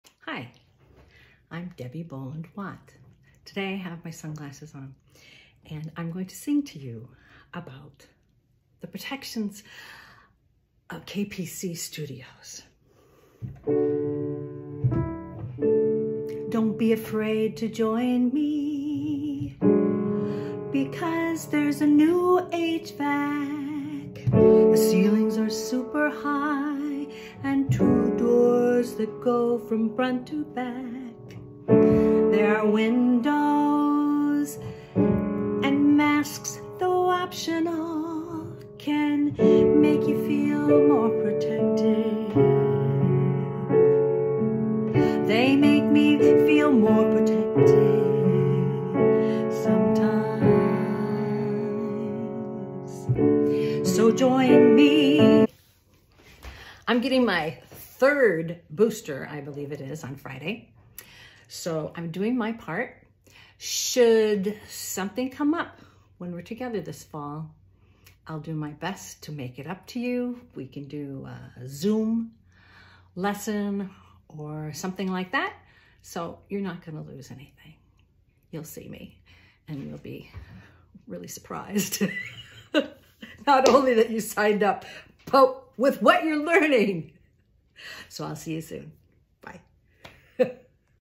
SYSA! Sing Your Soul Alive! at KPC Studios
Debby Boland Watt's NEW class offering at KPC Studios in Kirkland, WA (SYSA!) Sing Your Soul Alive! begins Wednesday, Sept. 13th.